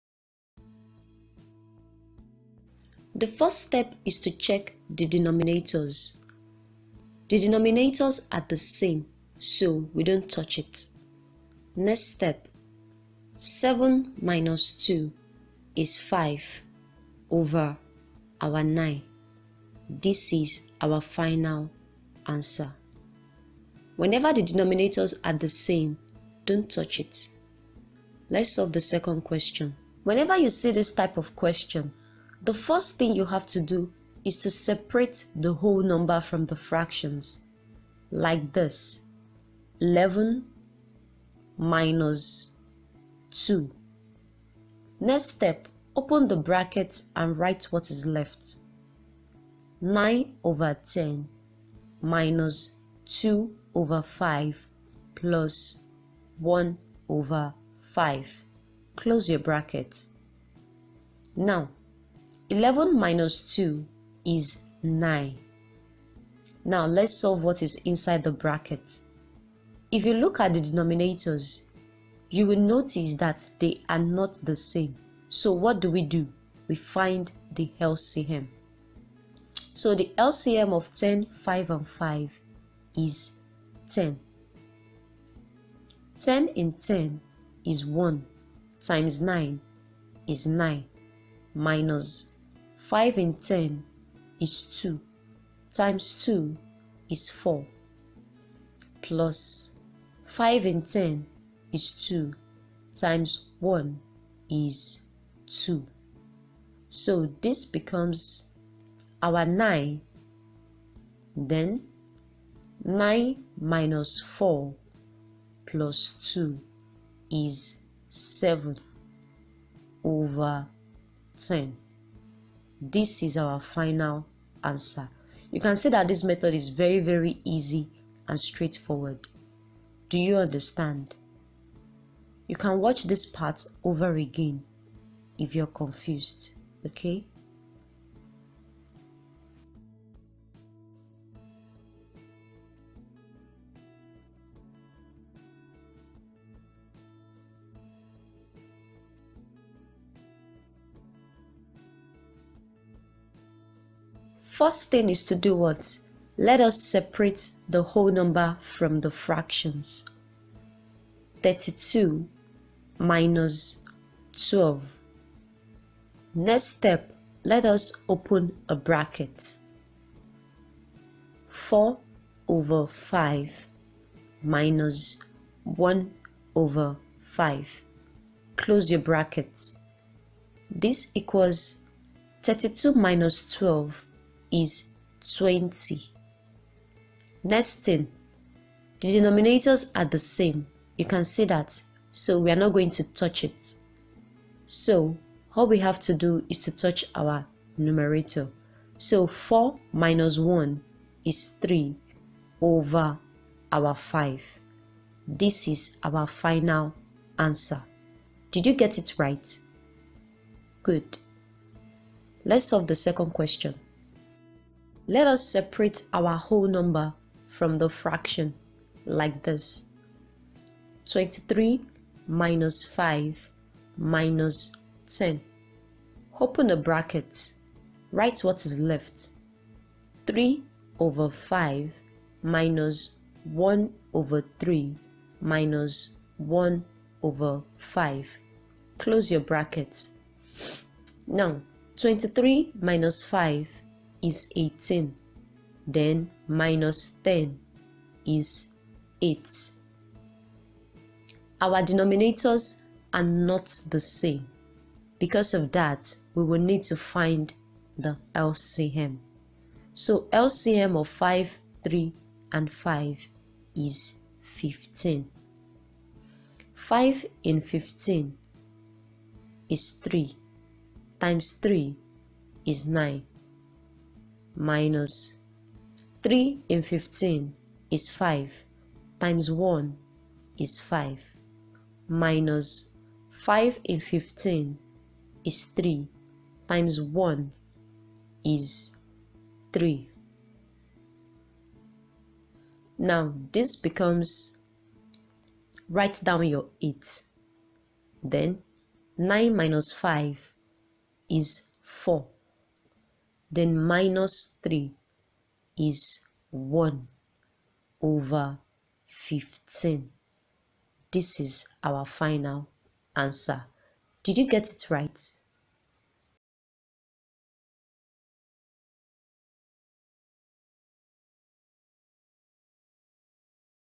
The Easiest Way to Subtract Fractions
Welcome!
In this video, you will learn how to subtract Fractions.
At the end of a lesson, you will be required to solve questions to test your understanding. Take your time, pause the video, solve the questions. When you are done, play the video to see the solutions.